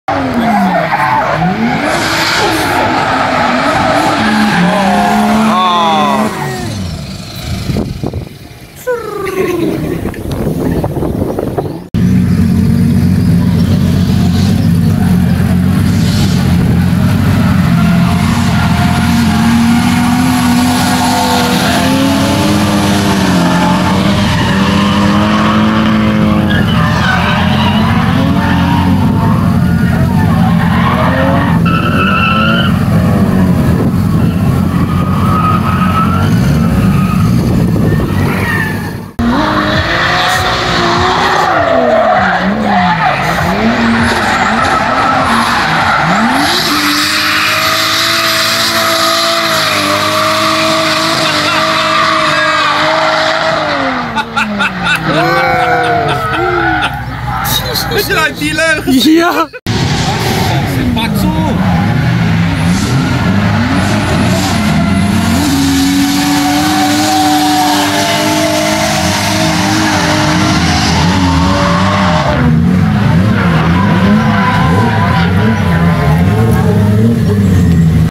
2JZ CRESSIDA SOLO RUNS (3 passengers)
I stopped by an event yesterday and did a couple of runs.
My buddy Ray got some clips :)

thumbnail photo: Danie van den Berg